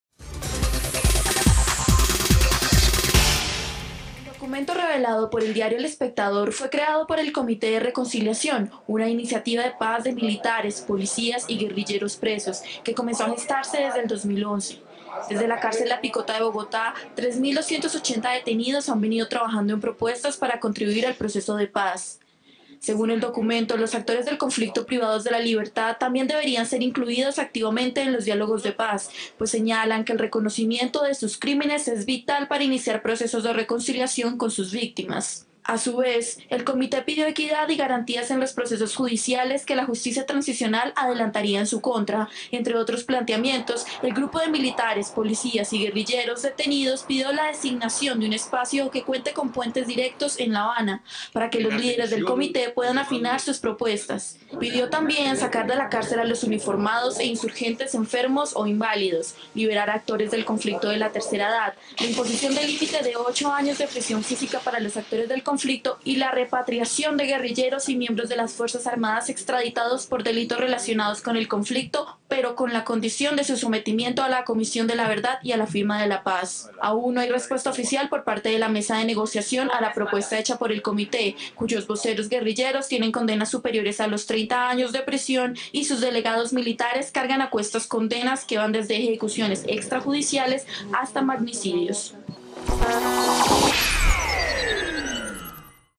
Militares y guerrilleros presos piden ser incluidos en la mesa de diálogos
En un comunicado que le hicieron llegar a los delegados del proceso de paz en La Habana, militares y guerrilleros presos solicitaron hacer parte de los diálogos que se llevan a cabo en la isla.